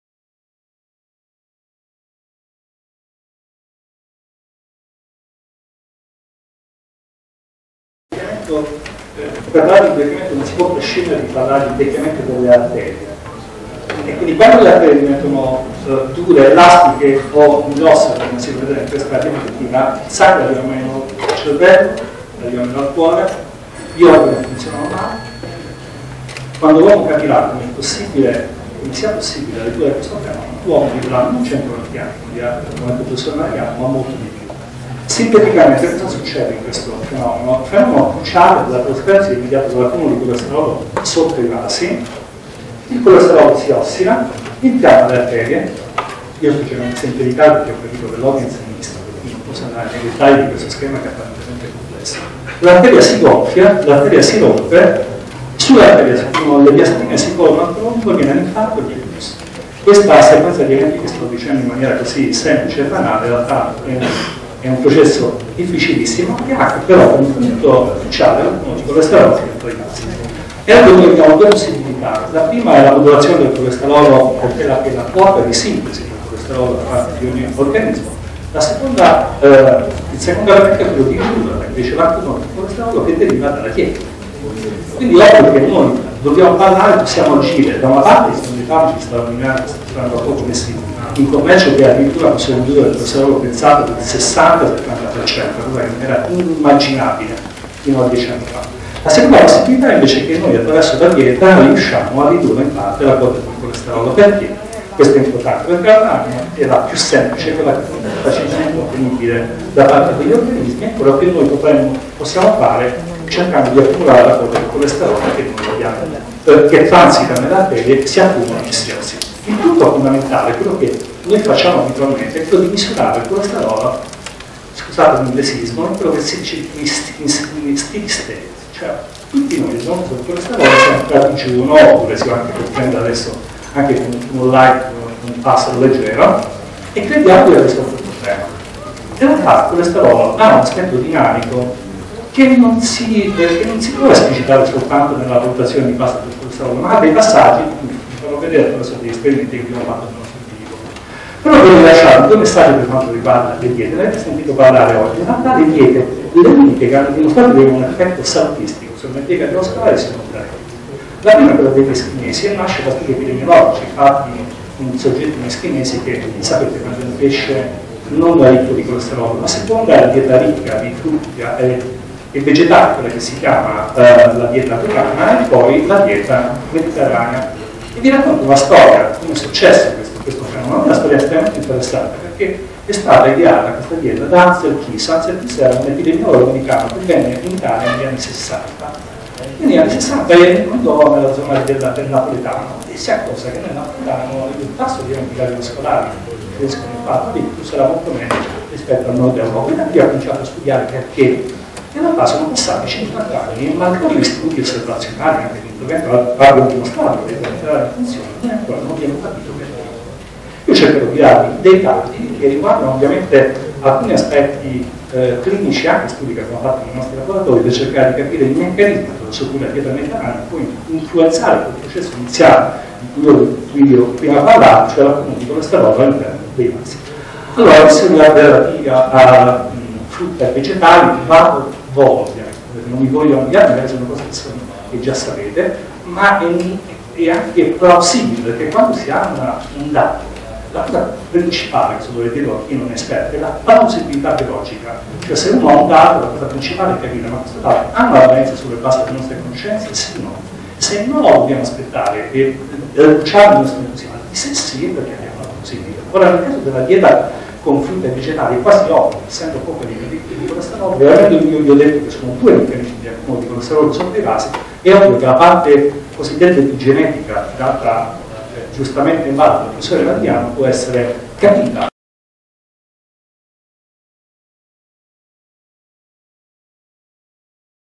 Francesco Violi - Simposio "Anziani una risorsa da valorizzare"
Registrazione effettuata da liberi.tv il 7/05/2015 a Roma presso Palazzo Ferrajoli.
Simposio: “Anziani, una risorsa da valorizzare” organizzato da Sara Iannone, presidente dell’associazione culturale “L’Alba del Terzo Millennio”.

Stralcio della relazione di FRANCESCO VIOLI, Direttore dipartimento Medicina Interna presso Umberto I - Policlinico di Roma.